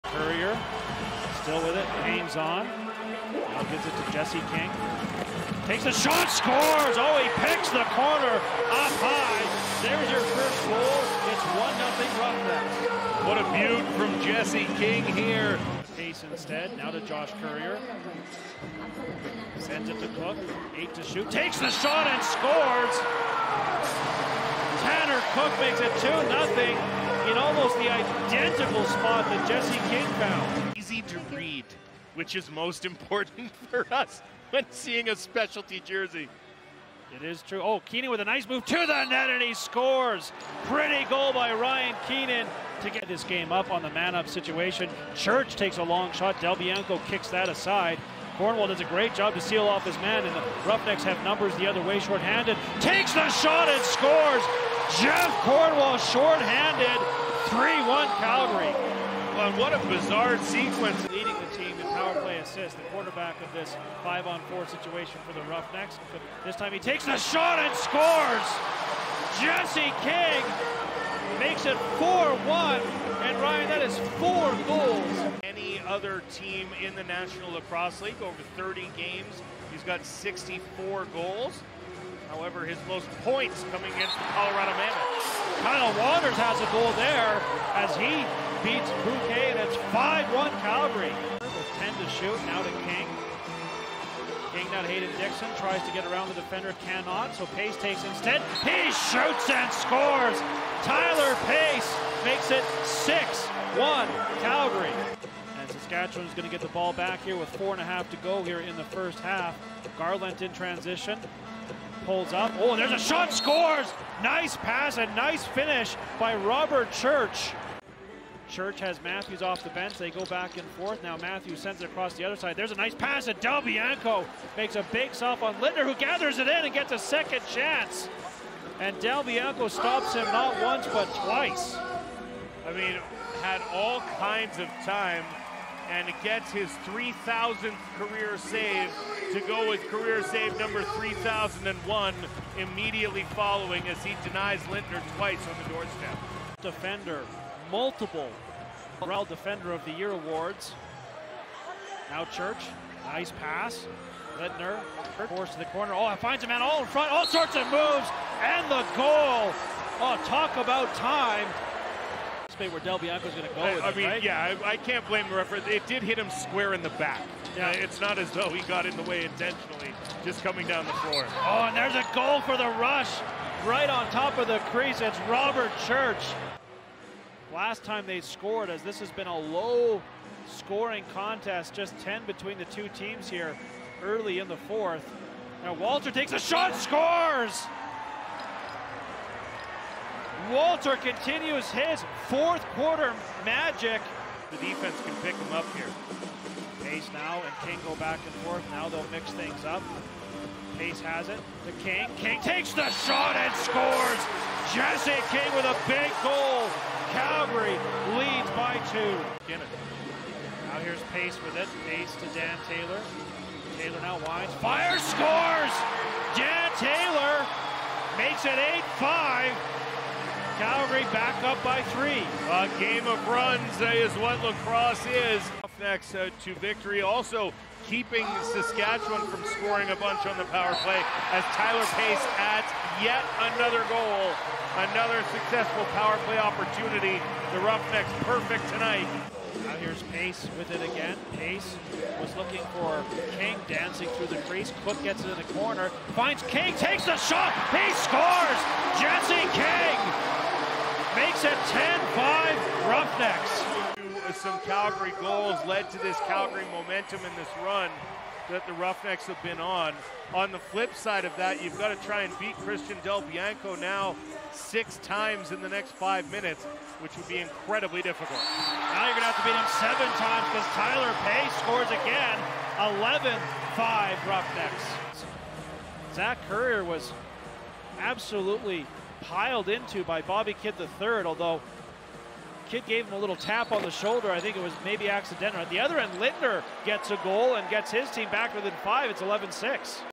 Full Game Highlights | Saskatchewan Rush vs. Calgary Roughnecks
Full highlights from the Calgary Roughnecks' win over Saskatchewan in front of over 17,000 fans at Scotiabank Saddledome. March 17, 2023.